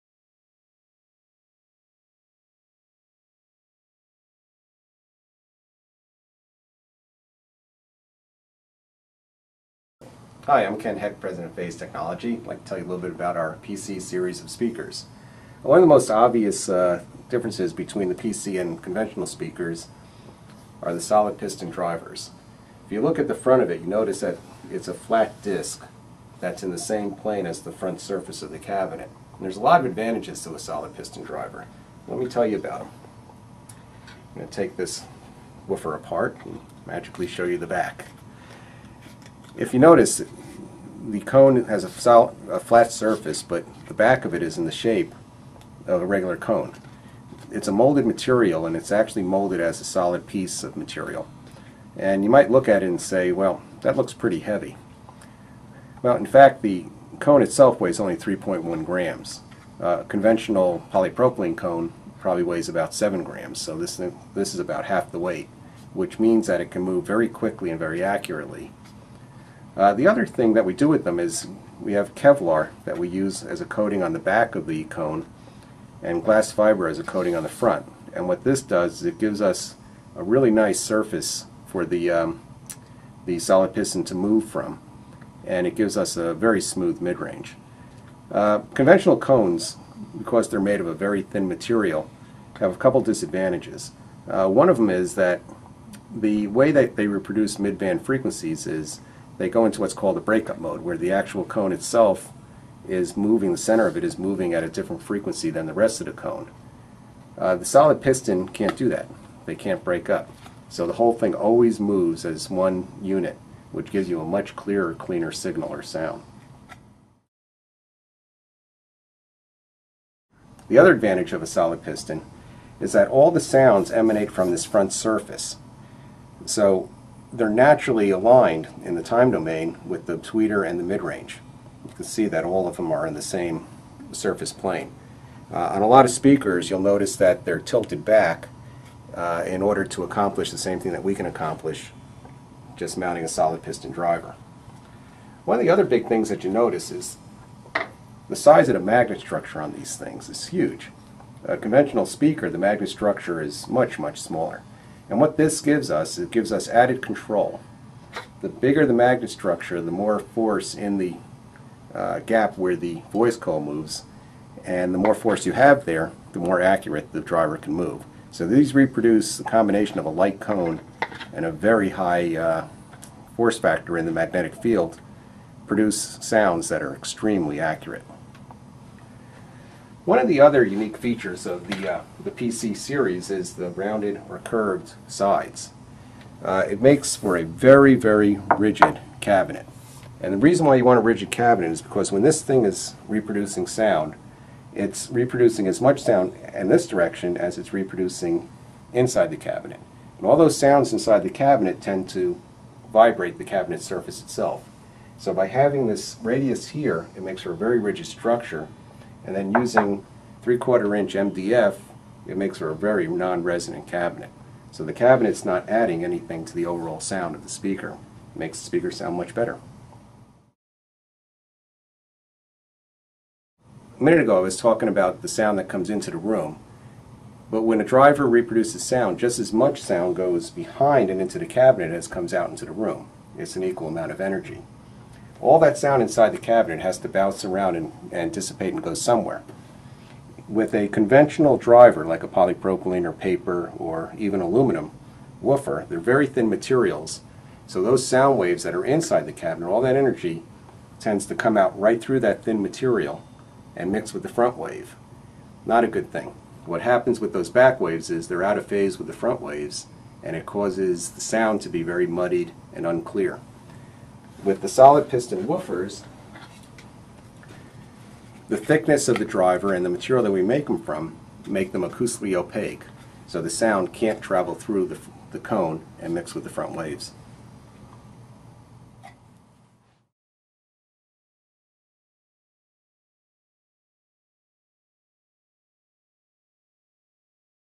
Phase Technology Flat-piston RPF drivers
The Premier Collection is the culmination of Phase Technology's over 50 years of research and manufacturing, designed specifically for dynamic, high performance home audio and home theater sound. The Premier Collection delivers unmatched realism by combining patented flat-piston RPF drivers, patented soft dome tweeters and a proprietary

Absolute Phase™ crossover, which creates a deep and balanced soundstage over the largest listening area of any speaker. Finished with real wood furniture-grade cherry veneers or nine layers of high-gloss piano black, these speakers look as good as they sound.